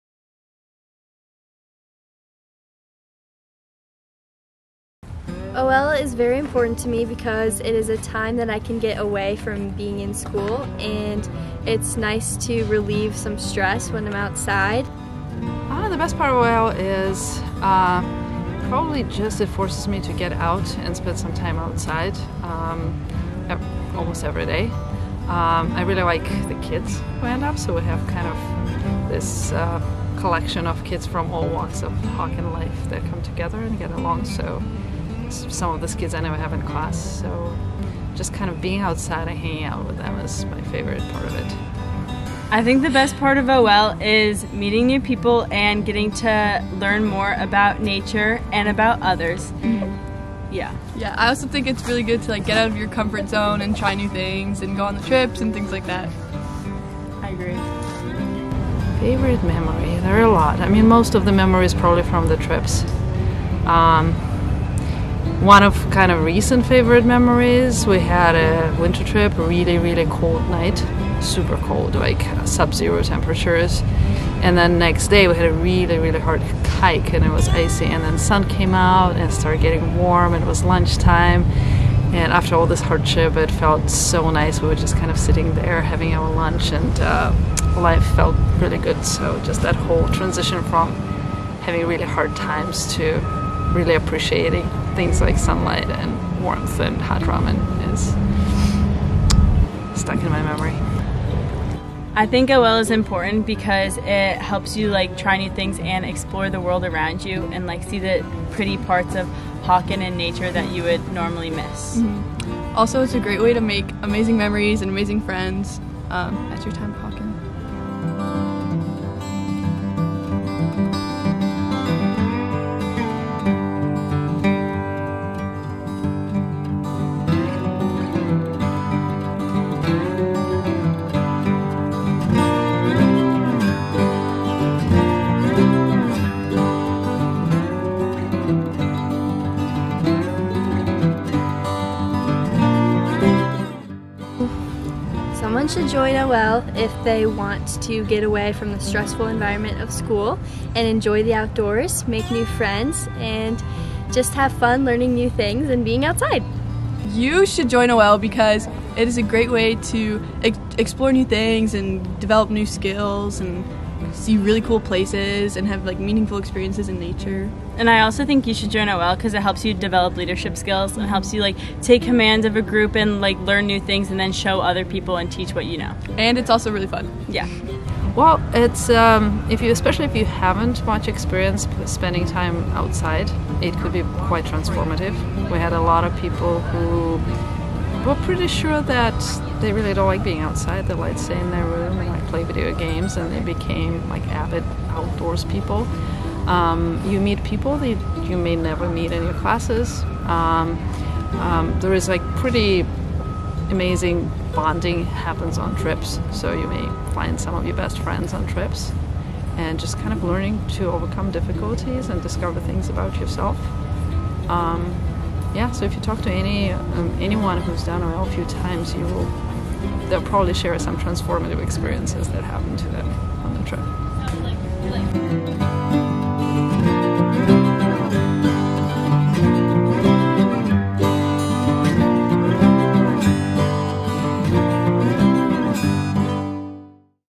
Outdoor Leadership at Hawken
A film about Hawken's Outdoor Leadership club by Hawken student Mila Grossman '24